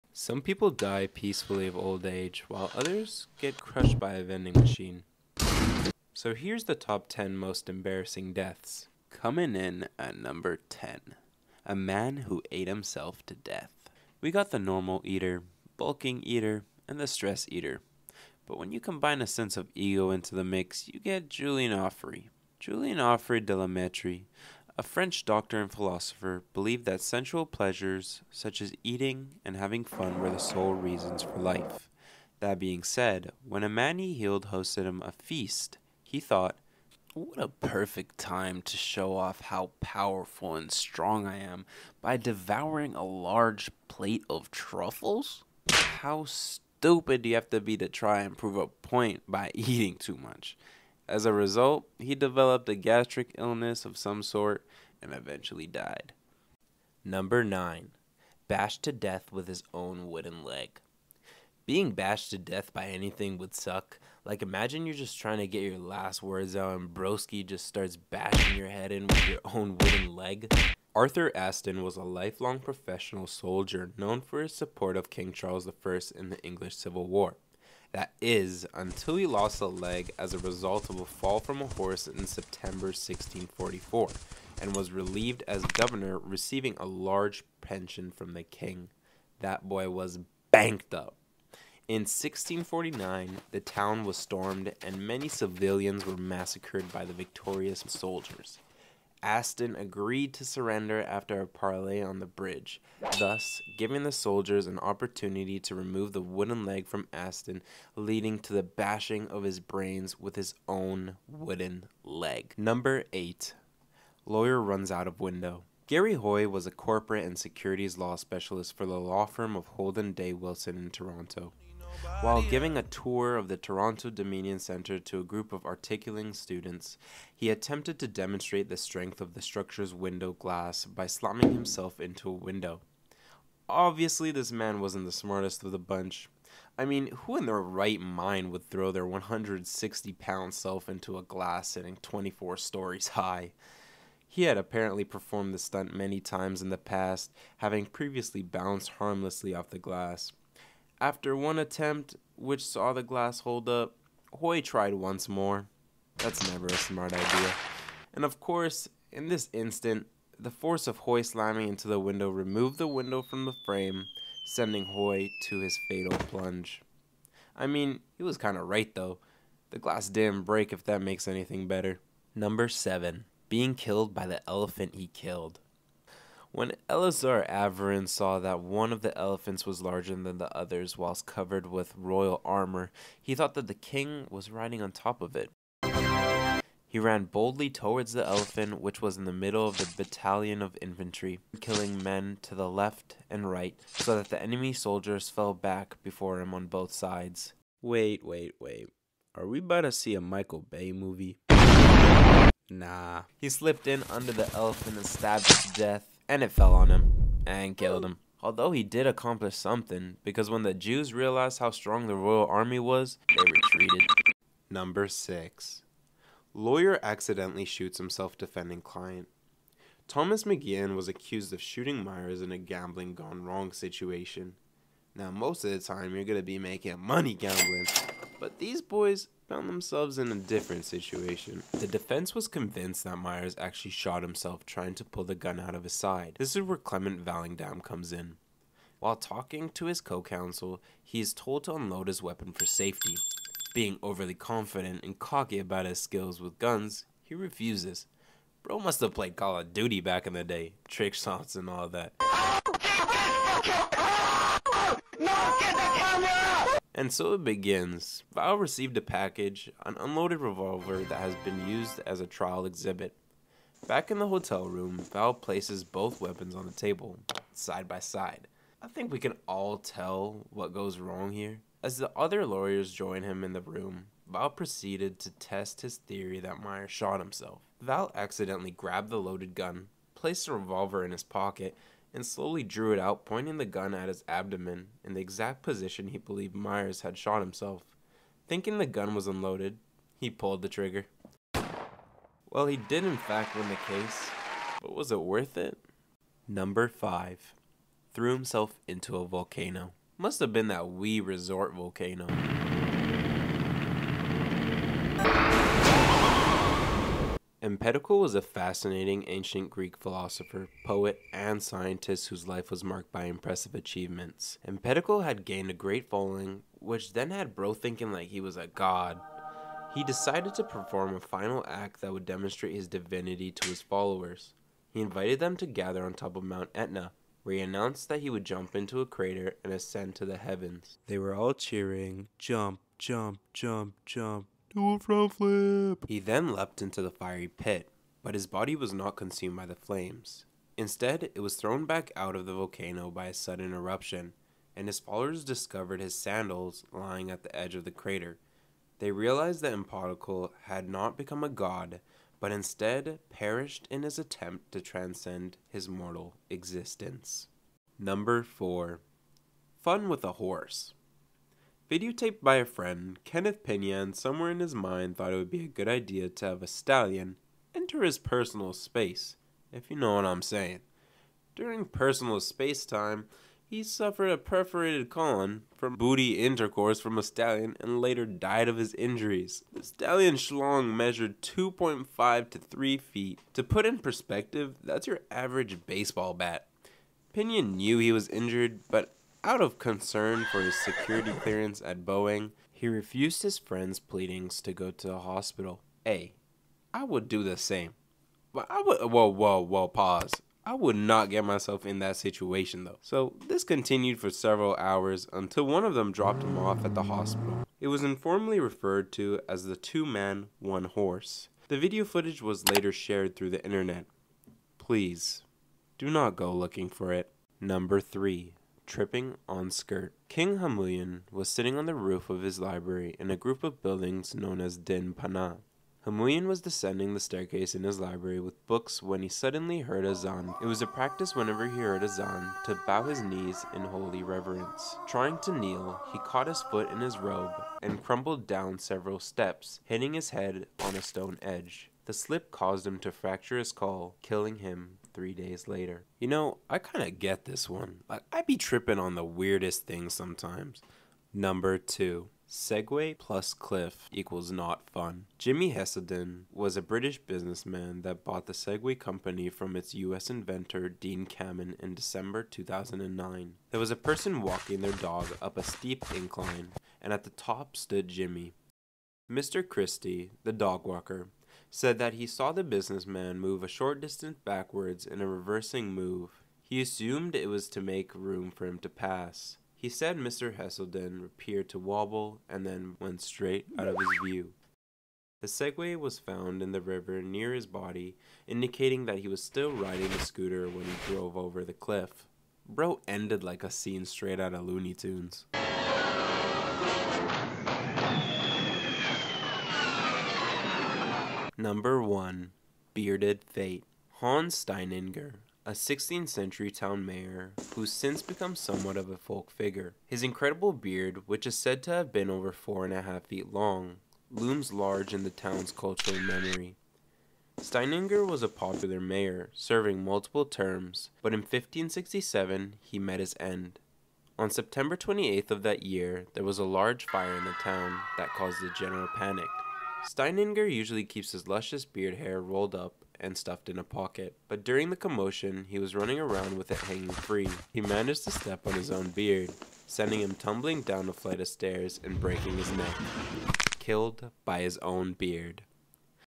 The 10 Most Embarrassing Deaths of All Time
Learn about the top 10 most embarrassing deaths
to ever happen to humans throught history.
From having intercourse with a horse, to getting beat with
your own leg these are 10 ways nobody would want to die.

inspired by: Good Enough

TimeStamps:
00:10 - Number 10
00:57 - Number 9
01:52 - Number 8
02:52 - Number 7
03:34 - Number 6
04:56 - Number 5
05:57 - Number 4
06:57 - Number 3
07:31 - Number 2
08:21 - Number 1

Trust me bro, Casual History, the paint explainer, Oh Boy!